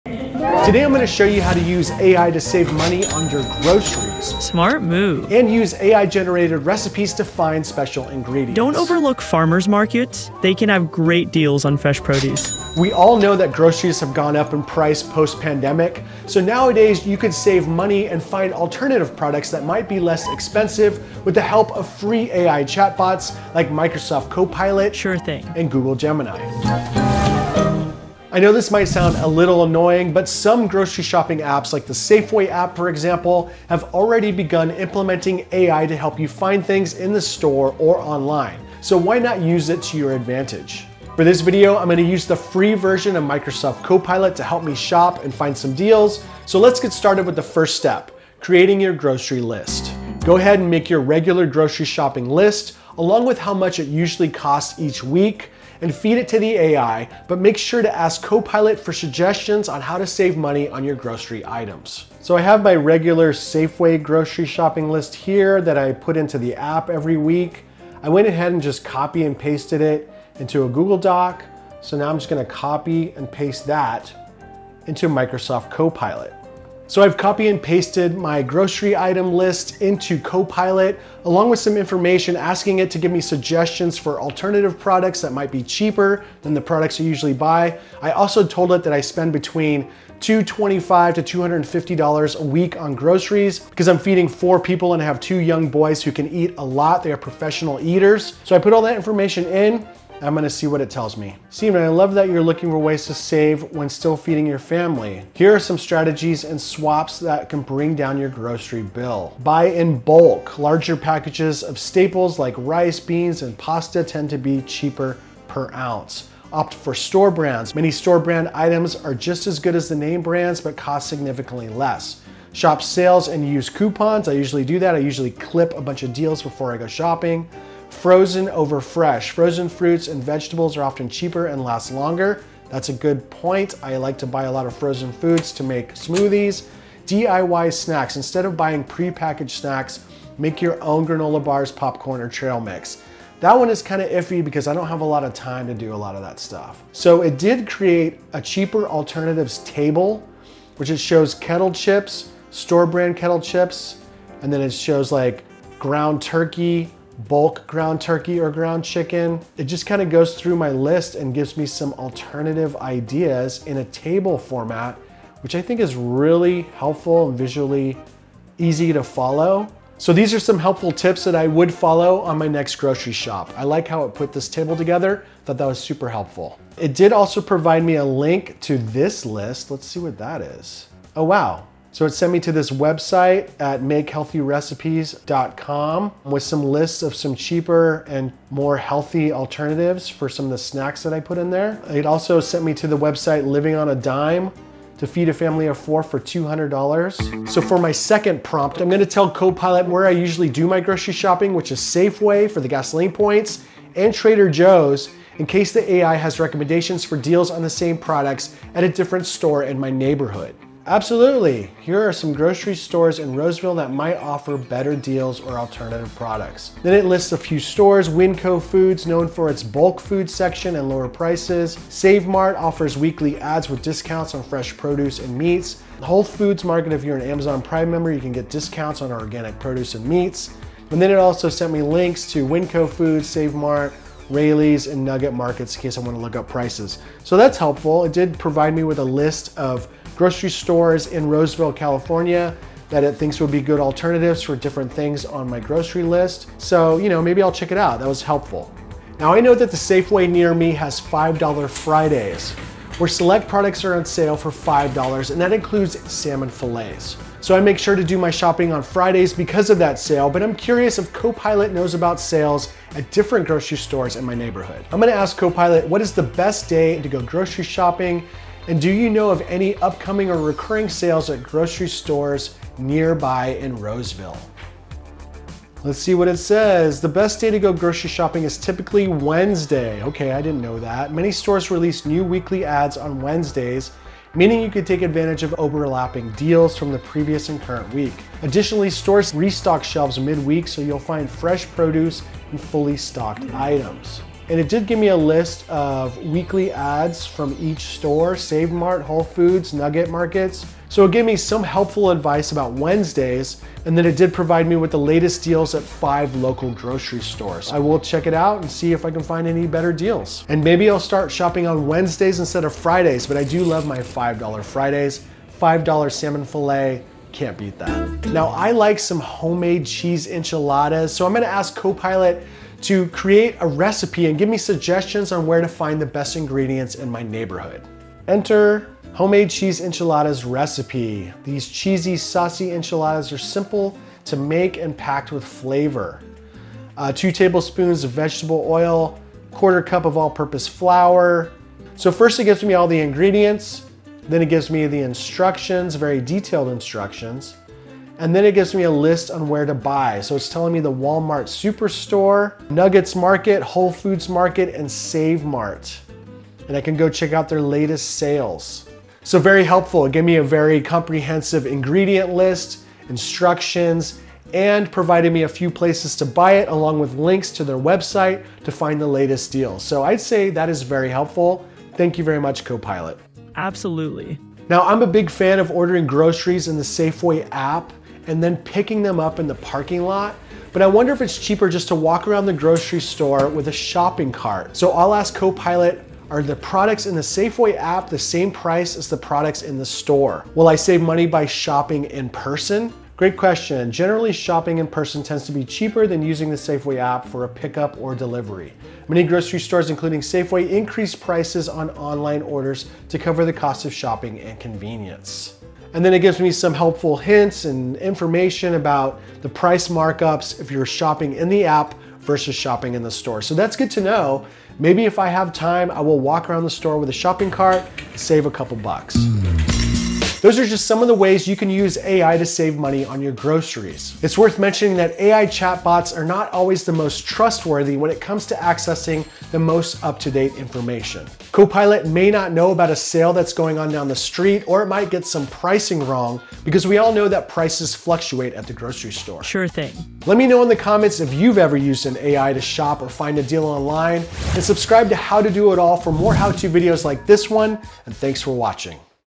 How to Use Microsoft Copilot to Save Money on Groceries
You can use free AI chatbots to save money on your grocery shopping list, create weekly meal plans and generate recipes complete with a list of ingredients and instructions. Here's how to get started with your AI shopping list.

Read more on how to save money on groceries with AI on CNET.com
How to Save Money on Groceries With the Help of AI

0:00 Intro
0:29 Use AI in Grocery Shopping Apps
0:46 Make a Grocery List
1:01 Copy and Paste Your Grocery List into Copilot
1:18 Ask Copilot How To Save Money and to Find Deals on Products on My List
1:41 Copilot's Advice Based on My Grocery List
3:17 Ask Copilot About Deals at Other Grocery Stores
4:20 Ask Copilot What's the Best Day to Go Grocery Shopping
5:18 Ask Copilot to For Recipes and to Find Deals on the Ingredients
6:15 Ask Copilot About Online vs. In-Person Shopping
7:00 Final Thoughts